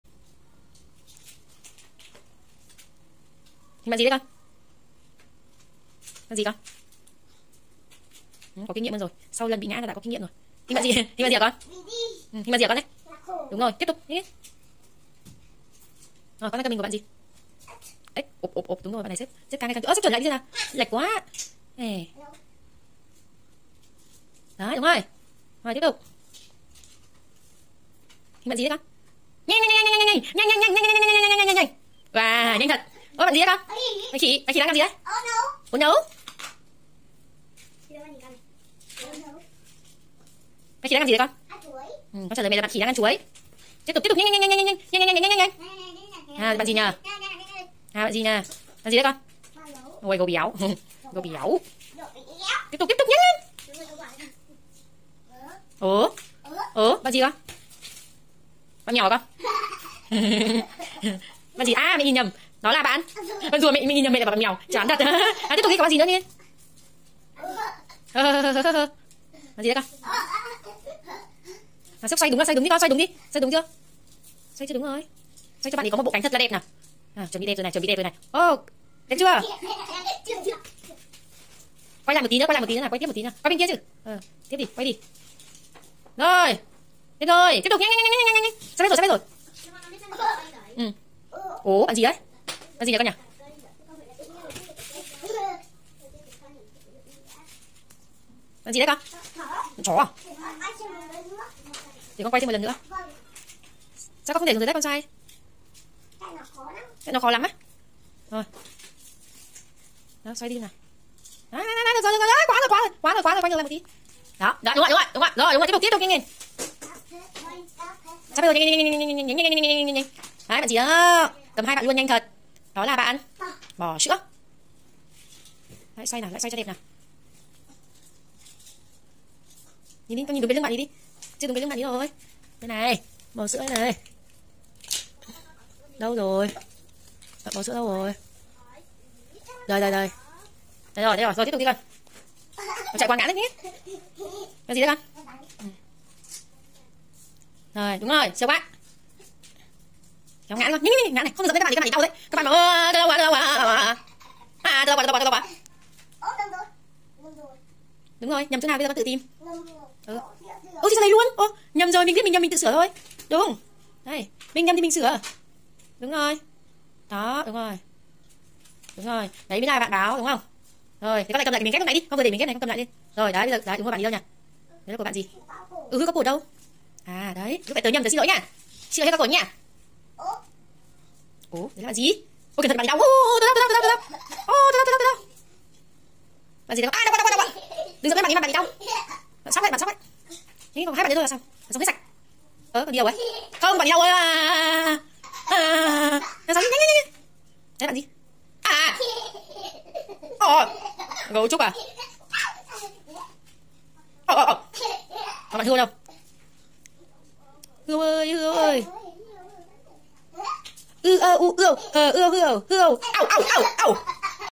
MATCHING TRANH | GHÉP HÌNH ĐỘNG VẬT dành cho bé 2-3 tuổi (có share file) (Phần 2)
Mình để lại link file để các mẹ có thể lấy về tự làm cho bé chơi nhé: